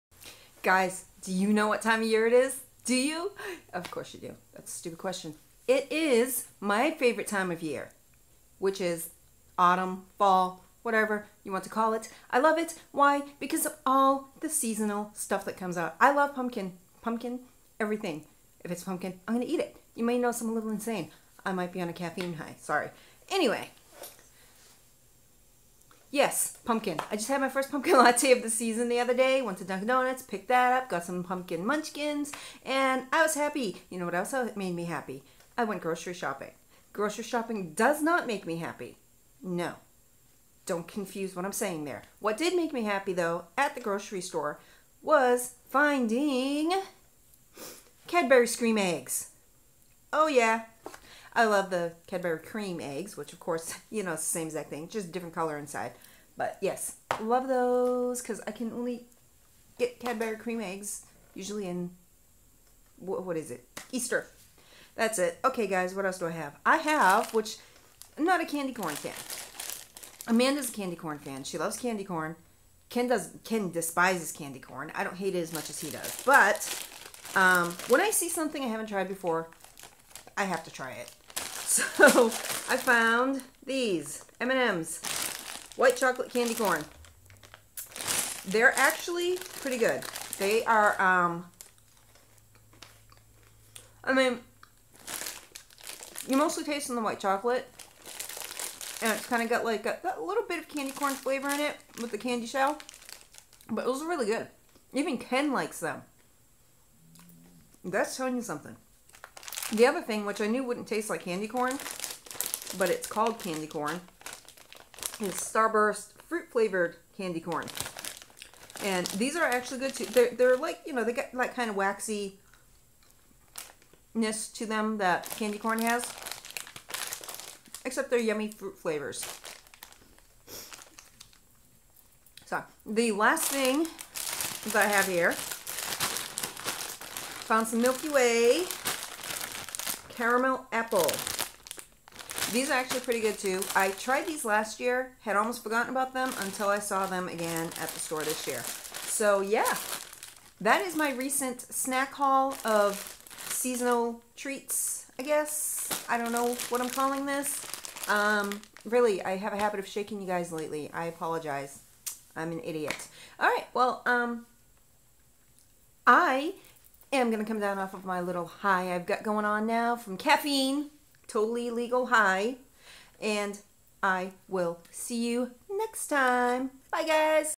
Snack Haul: Autumn Themed Treats
Yes. I have crazy eyes in this video. Also, towards the end, it sounds like I'm saying illegal, but it was actually "legal". heh I think I need to be kept away from the caffeine.